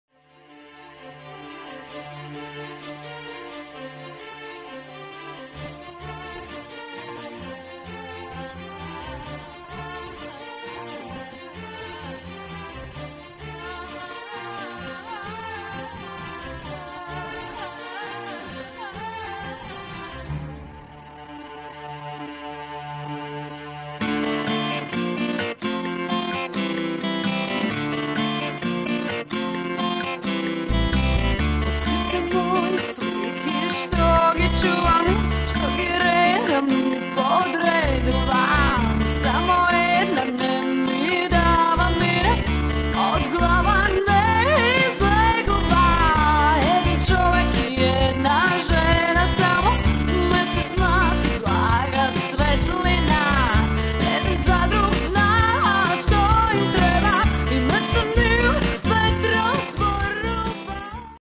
Wonderful Dancers from "DSC-BB" MACEDONIA IDOFEST Sarajevo 2015
DANCE SPORT CLUB "BRANIMIR i BRANKICA"
Production JAKO TEAM VIDEO
Support "The Trumpet Girls Group"